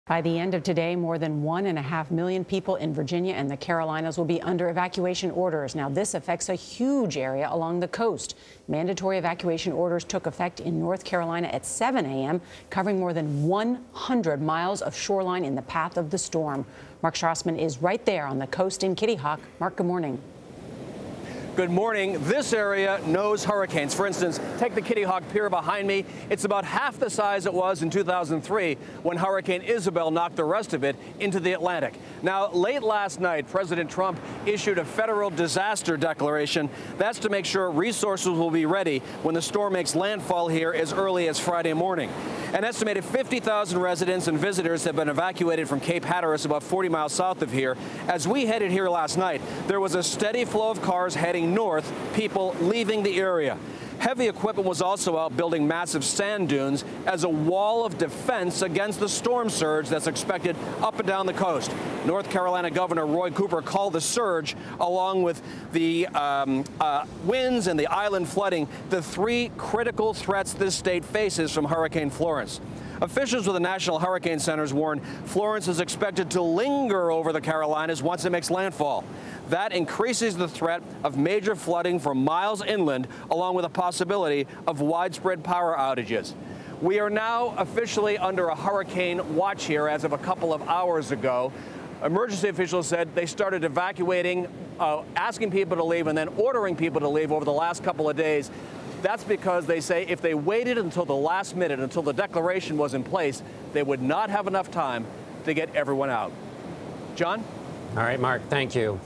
Hurricane Florence: Mandatory evacuations in North Carolina
By the end of Tuesday, more than 1.5 million people in Virginia and the Carolinas will be under evacuation orders. In Dare County, North Carolina, a mandatory evacuation order took effect at 7 a.m., covering more than 100 miles of shoreline in the path of the storm. Mark Strassmann reports.